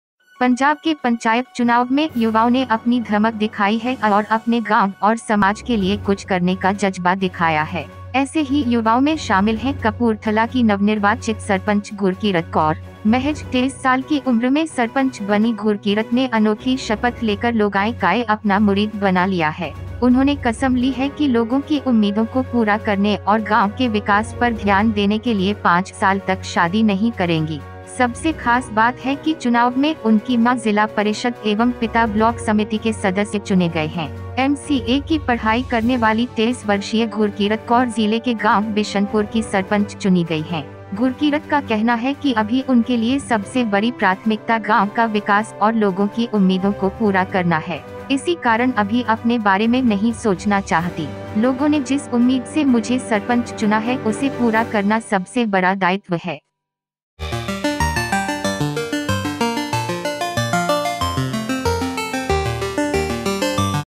23 साल की नव‍निर्वाचित सरपंच ने खाई अनोखी कसम, जिसने सुना हो गया मुरीद
23 साल की नव‍निर्वाचित सरपंच ने खाई अनोखी कसम, जिसने सुना हो गया मुरीद